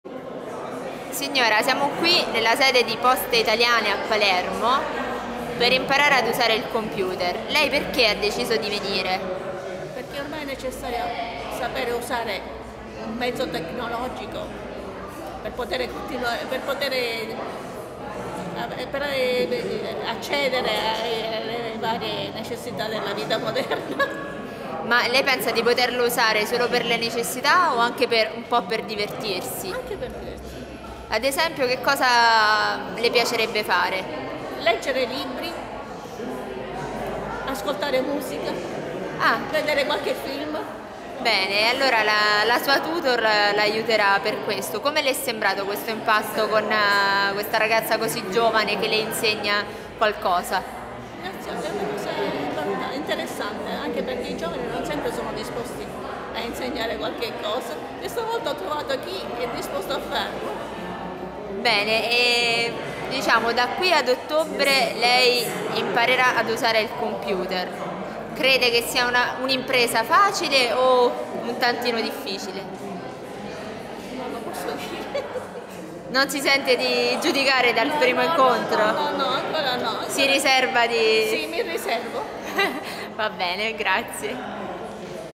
Nonni su Internet alle Poste con la signora Maria, 79 anni
Per aiutare le persone più anziane a navigare nel web e familiarizzare con le moderne tecnologie di comunicazione Poste Italiane ha scelto il modello di Nonni su Internet e con Fondazione Mondo Digitale promuove un progetto pilota di alfabetizzazione digitale presso gli uffici postali.